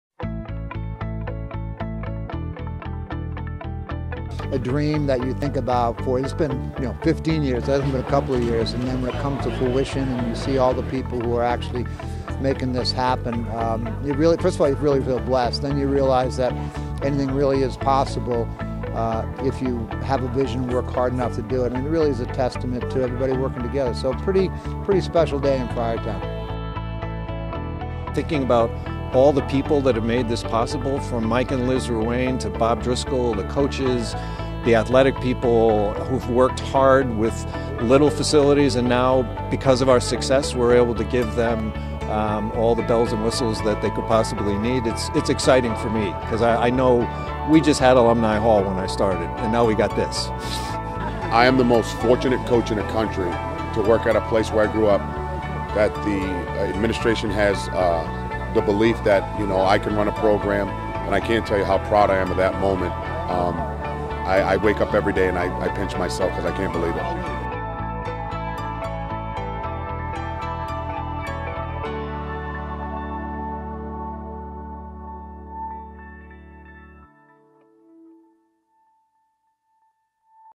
Ruane Topping
On February 15th, we put the final beam in place for the Ruane Friar Development Center. The completion of the building is anticipated to be Fall 2018.